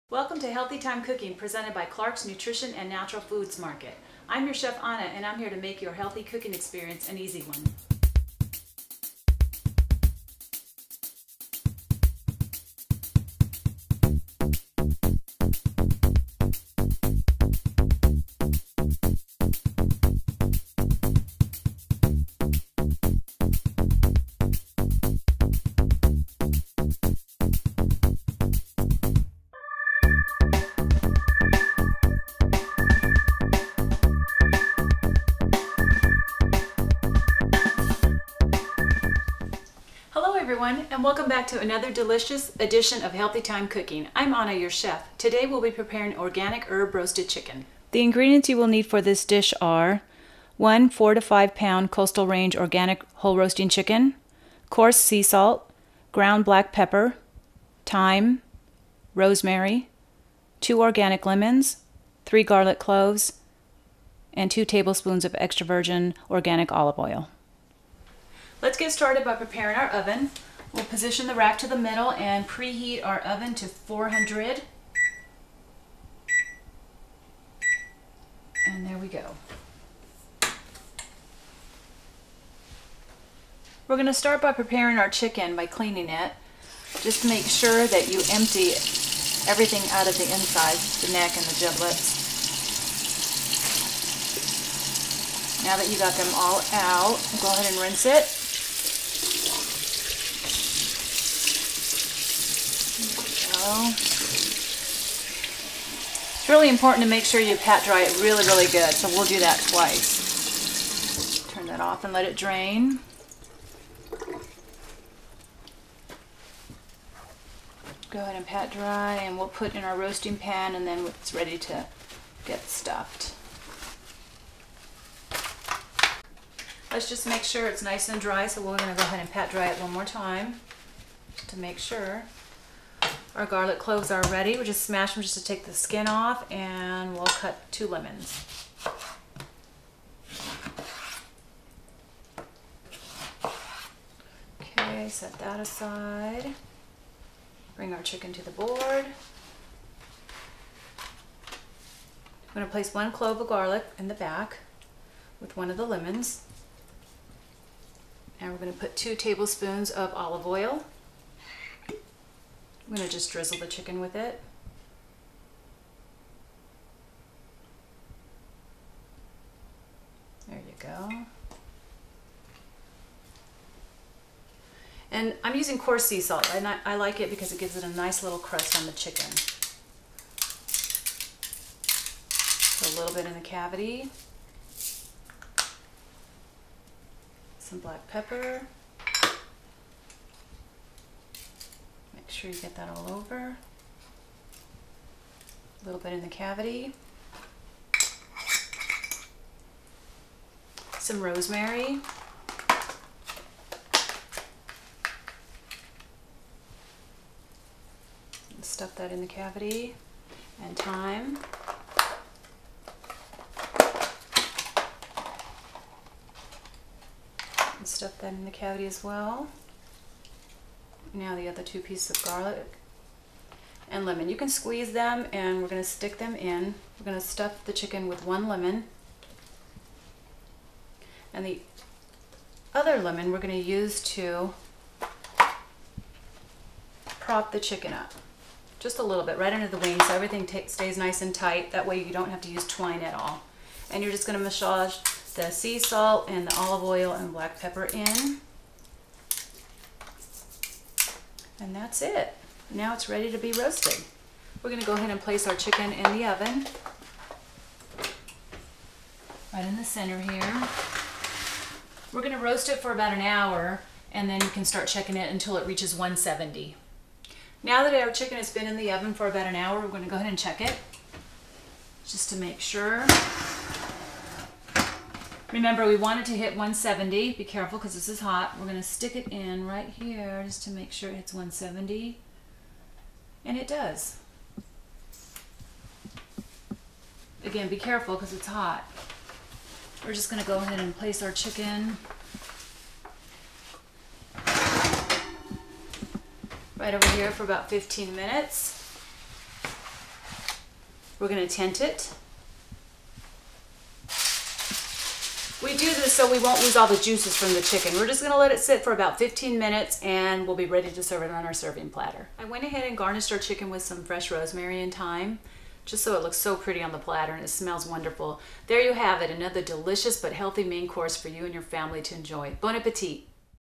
Clark's Nutrition-Organic Roasted Herb Chicken
Anna from Healthy time cooking prepares a delicious Organic Roasted Herb Chicken.